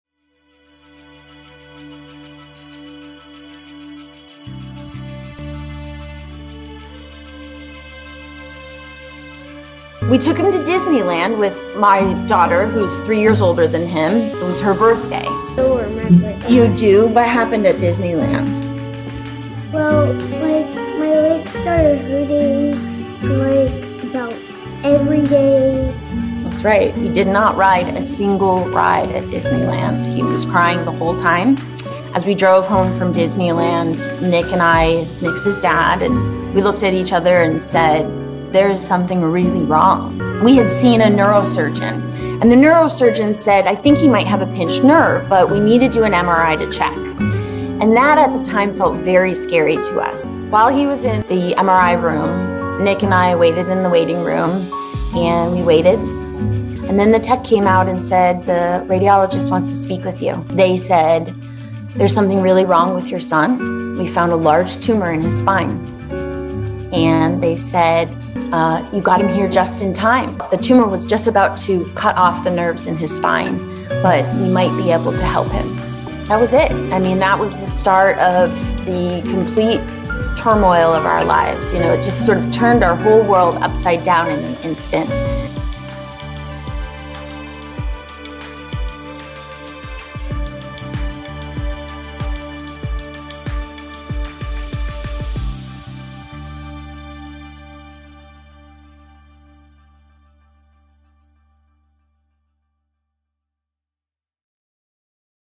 Meet Katie and Greyson Banovich at NFCR's 2022 Global Summit
Hear from this Mother and Son Duo about their journey from warning signs to cancer diagnosis and treatment to today.

Their cancer story is a story of hope. With more support for critical cancer research programs, we believe more cancer stories can have outcomes like their story.

Katie and Greyson will speak at the National Foundation for Cancer Research's 2022 Szent-Györgyi Prize Award Ceremony on October 22, 2022, at the National Press Club in Washington, D.C.

See how you can support their journey and this event